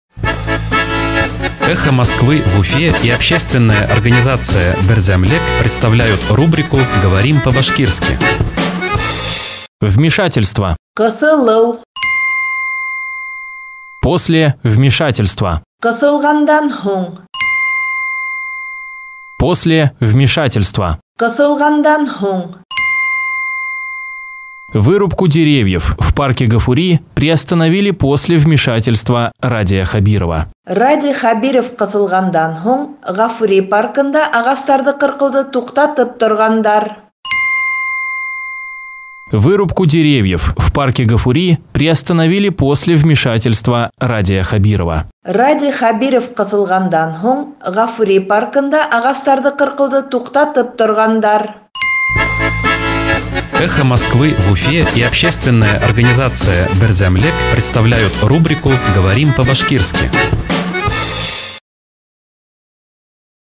Говорим по-башкирски: «Вмешательство» – "Ҡыҫылыу" от 12 ноября 2020 года
Вмешательство. После вмешательства. Вырубку деревьев в парке Гафури приостановили после вмешательства Радия Хабирова.

Ҡыҫылыу. Ҡыҫылғандан һуң. Радий Хәбиров ҡыҫылғандан һуң Ғафури паркында ағастарҙы ҡырҡыуҙы туҡтатып торғандар.